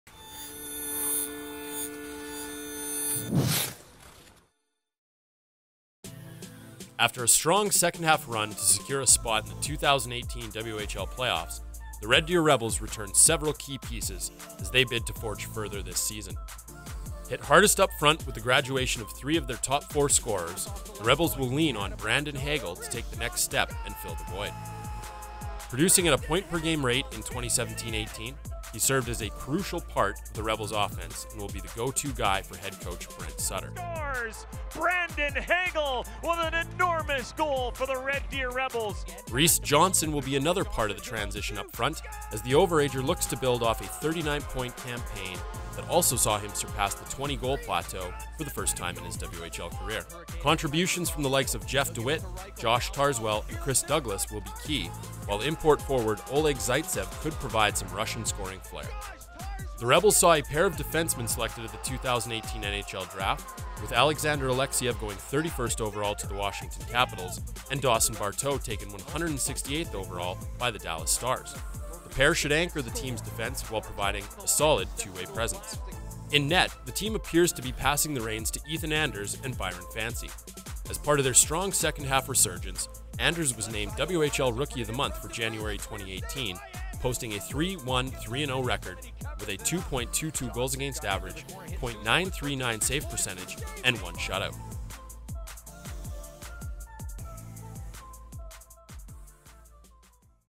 SEASON PREVIEW | Red Deer Rebels | 2018-19
Losing so many key members up front, it’s tough to predict where the Rebels will finish. Following last season’s rough first half, the returning players will be eager to change the script at the start of the season this time around and not have to put pressure on themselves like they did during the second-half charge back into the playoff picture. With Sutter at the helm, expect the Rebels to continue their annual tradition of being a tough opponent in the Central Division.